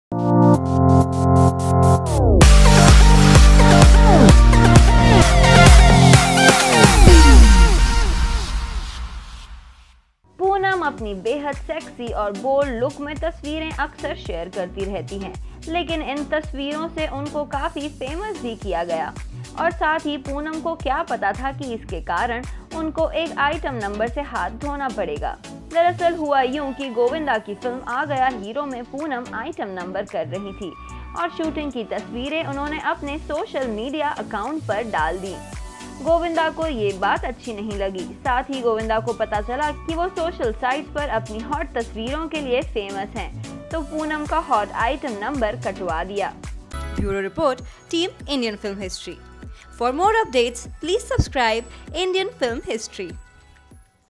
फिल्म में से कैसे कटा Poonam Pandey का आइटम सॉंग | IFH
Poonam pandey bothered govinda so much that he had to take such steps

Developed and Managed By:

1/3,Shere Punjab Colony,
Andheri East.
Mumbai 400 093
India.